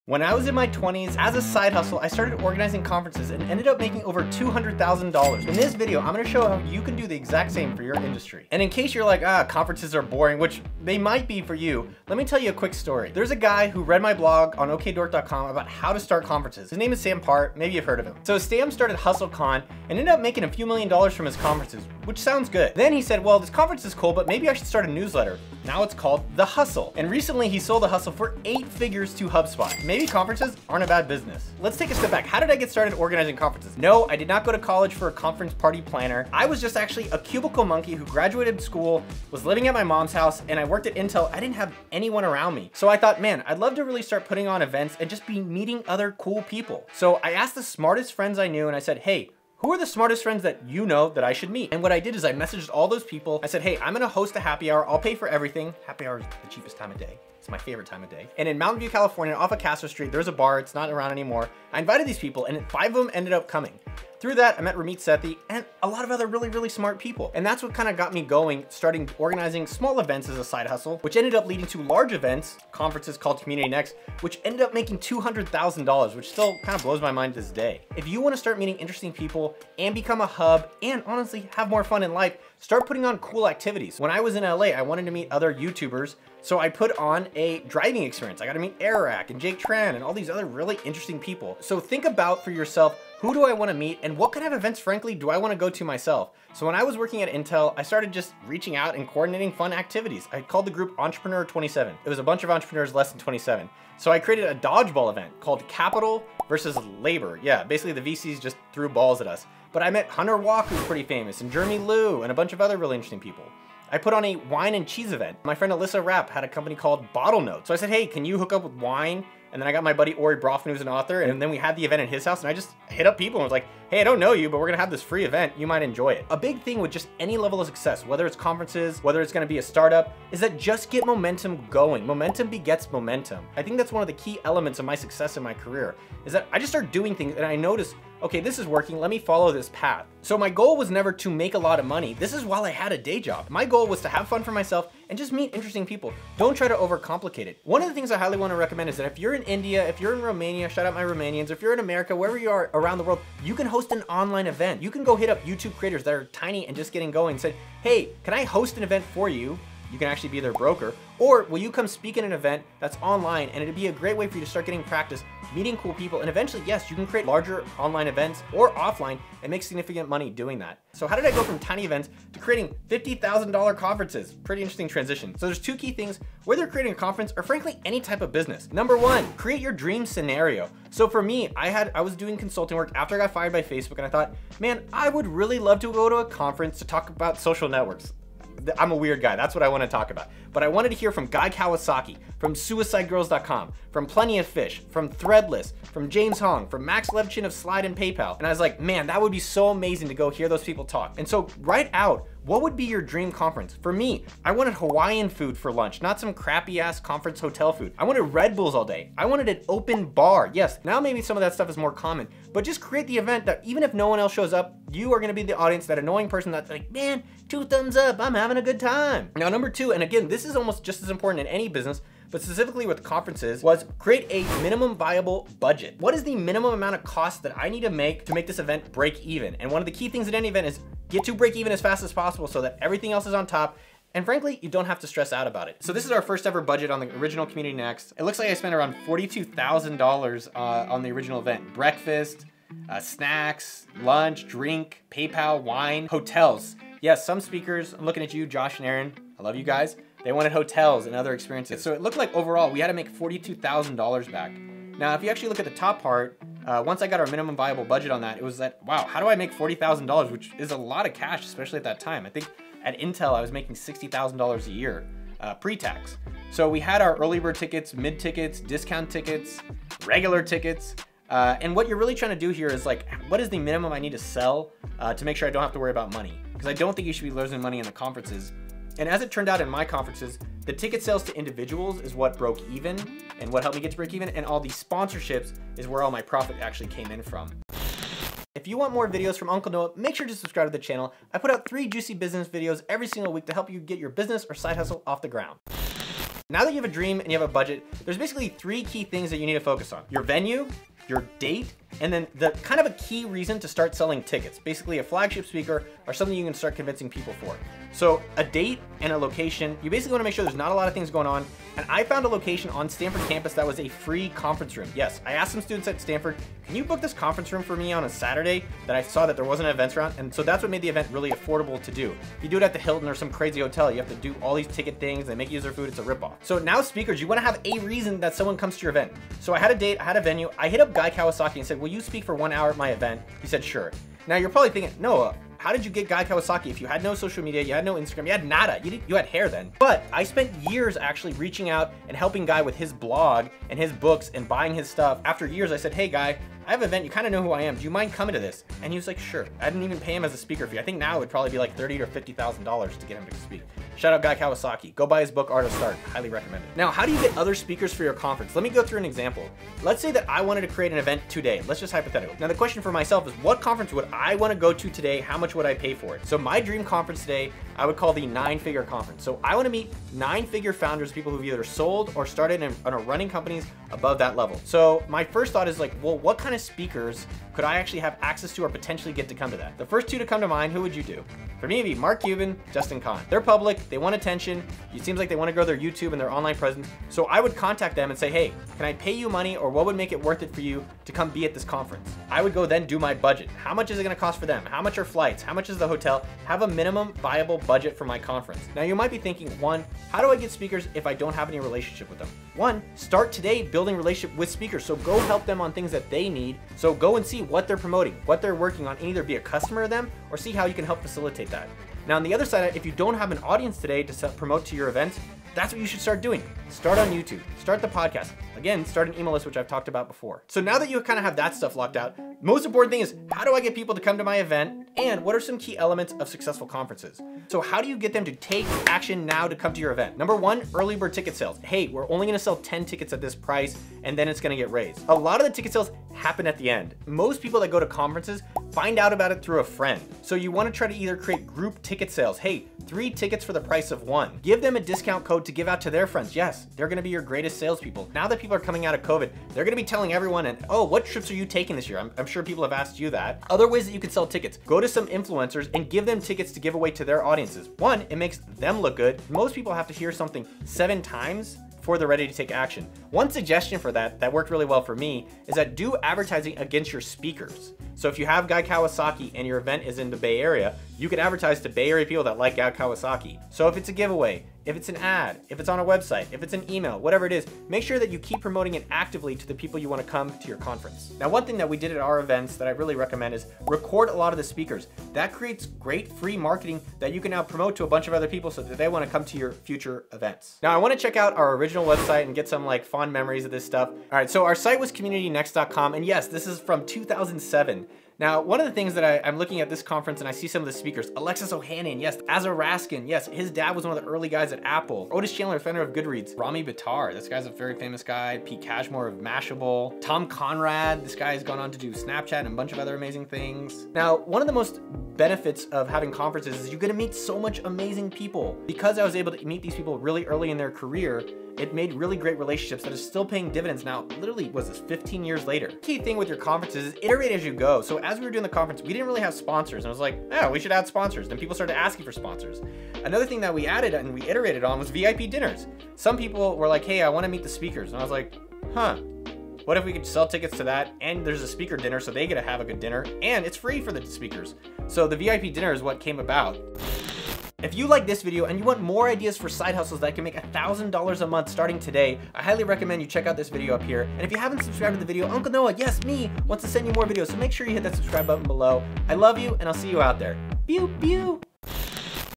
How I Made $200,000 Organizing Conferences (Best Side Hustle)
When I was in my twenties, I made over $200,000 organizing conferences, and in the process I became friends with some of the most influential people in Silicon Valley.

In this video I’m going to show you how you can do the same for your industry!

📈 Join AppSumo’s free newsletter to get the BEST software tools and deals for your business

📸 Connect on IG. I reply to DMs.

Bookmarks:
0:00 Intro
0:15 How I got started with throwing conferences
2:50 How to make $50k conferences
4:55 Pro Tips for organizing a conference
5:55 How to get speakers
7:02 How to market your event
8:10 Communitynext example